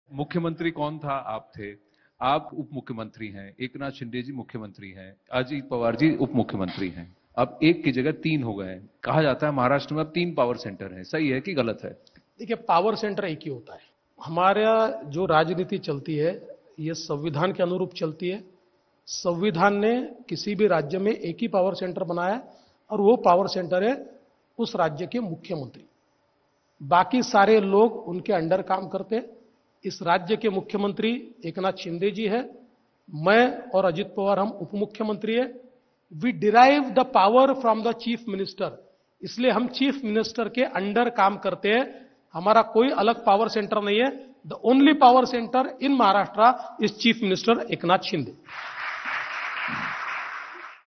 महाराष्ट्र में सिर्फ एक Power Center... | मुंबई | उप मुख्यमंत्री देवेंद्र फडणवीस
(बदलाव हमसे है - न्यूज18 लोकमत | मुंबई | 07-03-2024)